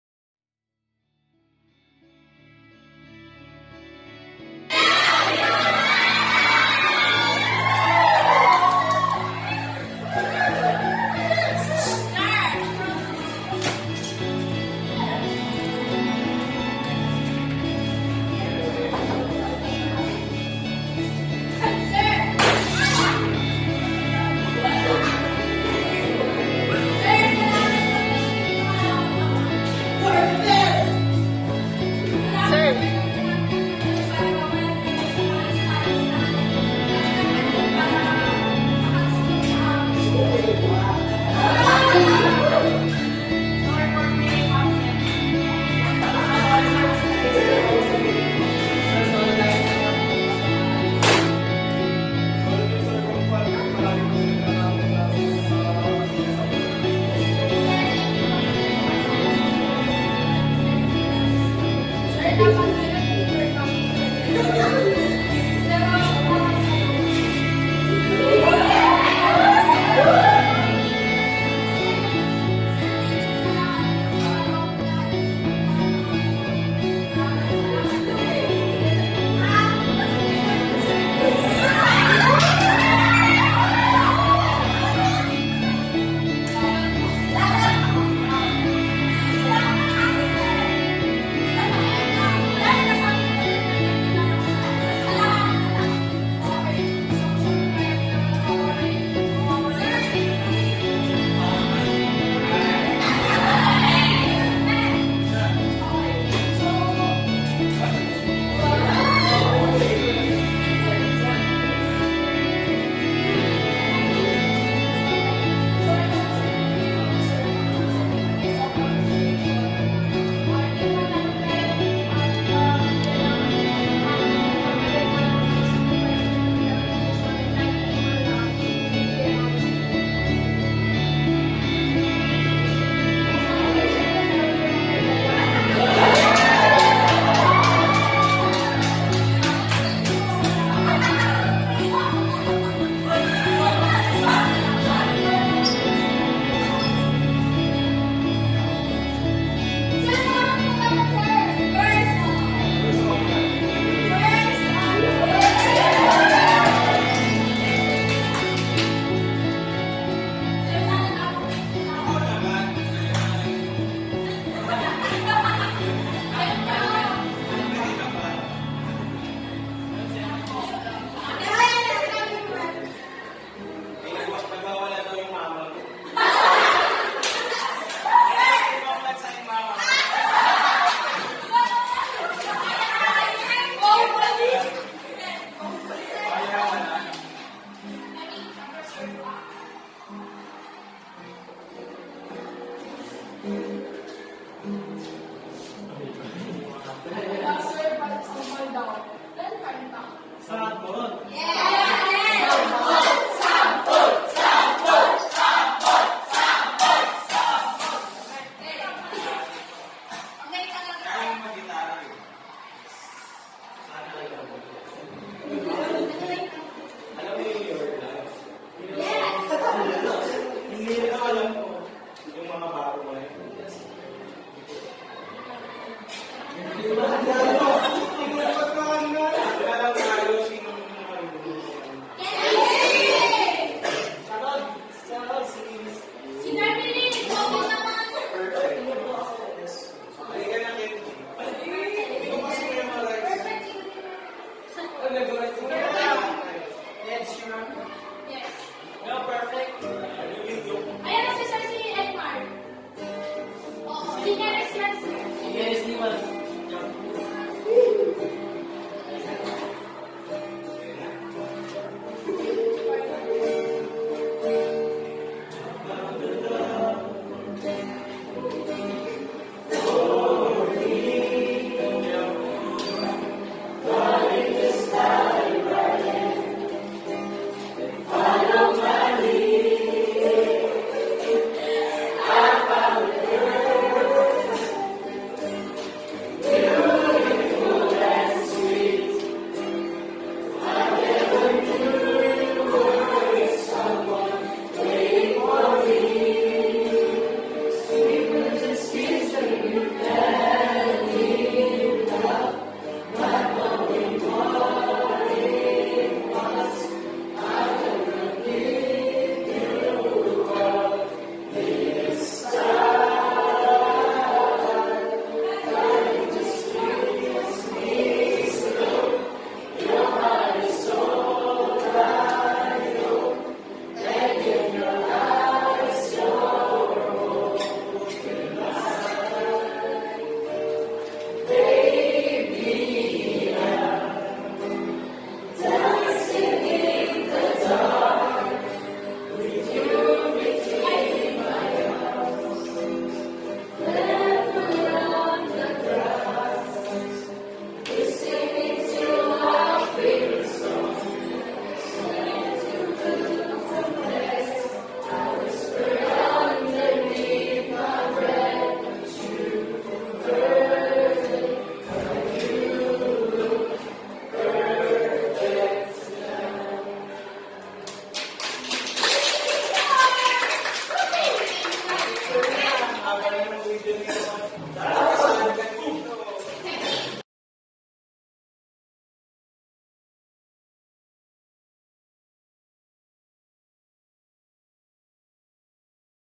Happy Valentines sir 💓
Thank you for everything sir di namin kakalimutan lahat ng payo m sa Amin 💓lablab ka namin ng buong klase mo sir Happy Valentines po ulit sir Godbless.